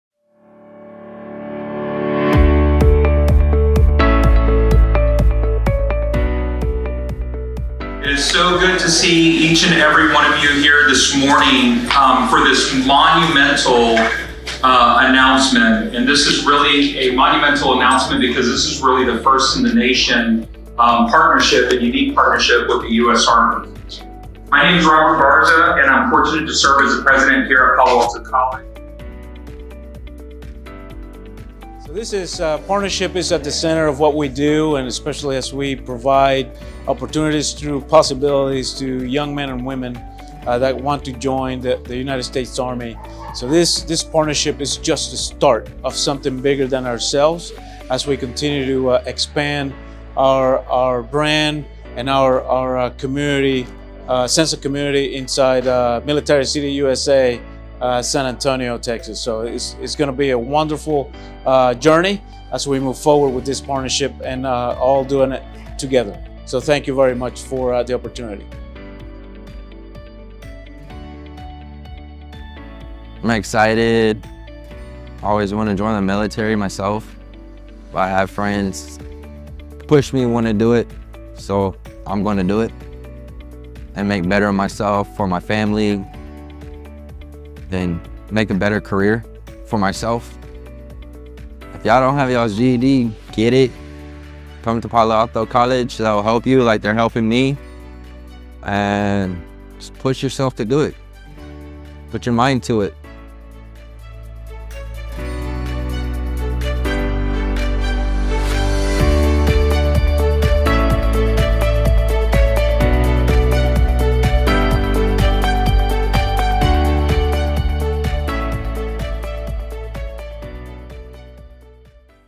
Palo Alto College Future Soldier Signing Ceremony
We are proud to announce that Palo Alto College has launched an innovative partnership with San Antonio's U.S. Army recruitment offices by welcoming our first two students to our Future Soldiers Program during an official signing ceremony on July 10, 2024 in the Ozuna Legacy Room. 📝👏

This program allows prospective soldiers to initiate their GED registration through PAC at any of the city's five Army recruiting stations. It will help guide these individuals with educational access and military readiness, promising to set new standards for career preparation in Military City San Antonio. 💫

To learn more about the Future Soldier Program, call Taylor Coffee, Coordinator of High School Programs, at 📱 or email him at 📩 .